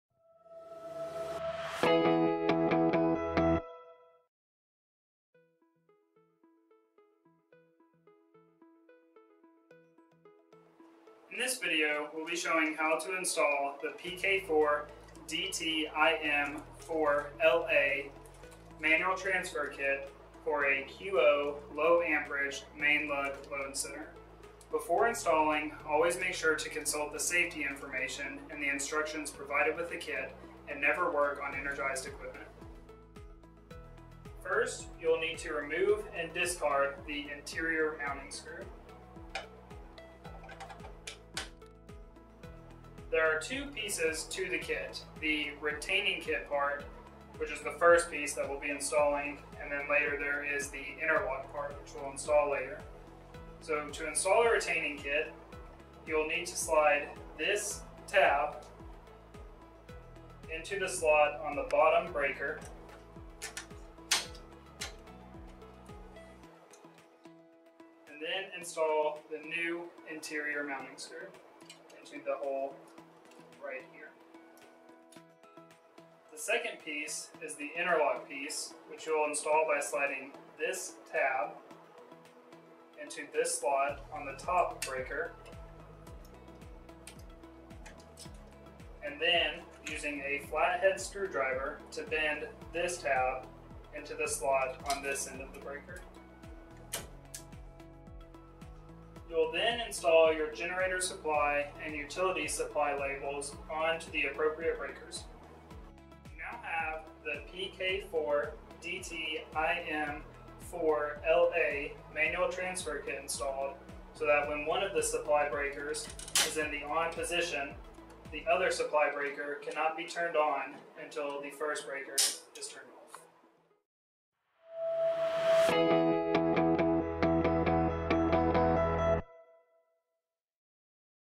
Installing PK4DTIM4LA Manual Transfer Kit into Low Amp QO™ Load Centers | Schneider Electric Support
Tutorial for installing the PK4DTIM4LA Manual Transfer Kit into single-phase, convertible mains, 125A max rated Low Amp QO™ Load Centers. PK4DTIM4LA is used to secure two adjacent, QO™ 2-pole branch circuit breakers WITHOUT electrical accessories in positions 2-4 and 6-8 (right side) of the interior when used as a Back-Fed Main Breakers.

NOTE: This video is a supplement to the instructions included with the equipment. Please read and understand these instructions before working on any electrical equipment.